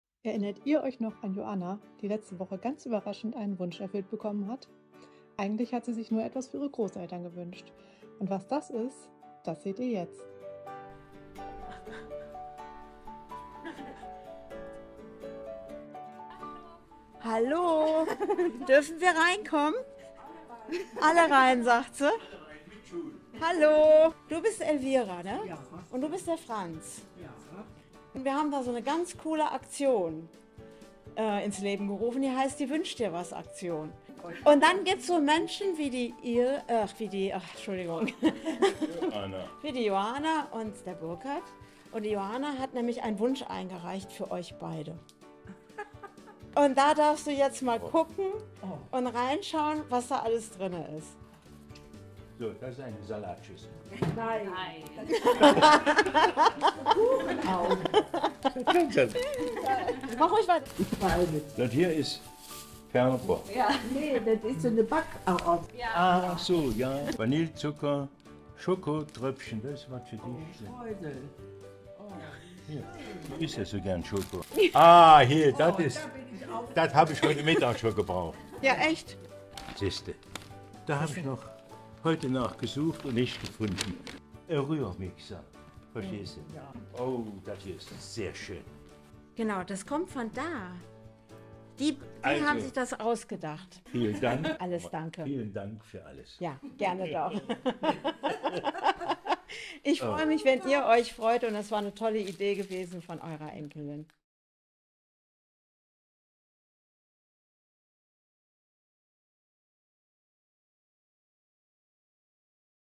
Überrascht von der Enkelin!
Joanas Großeltern, die in der verheerenden Flutnacht alles verloren haben, wurden von ihrer Enkelin mit einem bewegenden Wunsch überrascht. 🎁 Joana, tief berührt von dem Leid, das ihre Großeltern erfahren hatten, reichte einen Herzenswunsch ein, um ihnen einen Funken Lebensqualität zurückzugeben. ✨
Dieser Wunsch ermöglichte es ihnen, nach den schweren Verlusten wieder einen Hauch von Normalität zu erleben. Die Freude und Dankbarkeit in den Augen der Großeltern bei der Erfüllung des Wunsches spiegelten die tiefe Verbundenheit und Liebe innerhalb der Familie wider. ♥️
Wir können die Betroffenen weiterhin auch vor Ort unterstützen. Sei dabei und melde dich an!

Das ist durch @helpfromgermany möglich, welche diese Aktion seit dem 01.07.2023 finanzieren! 🙏🏼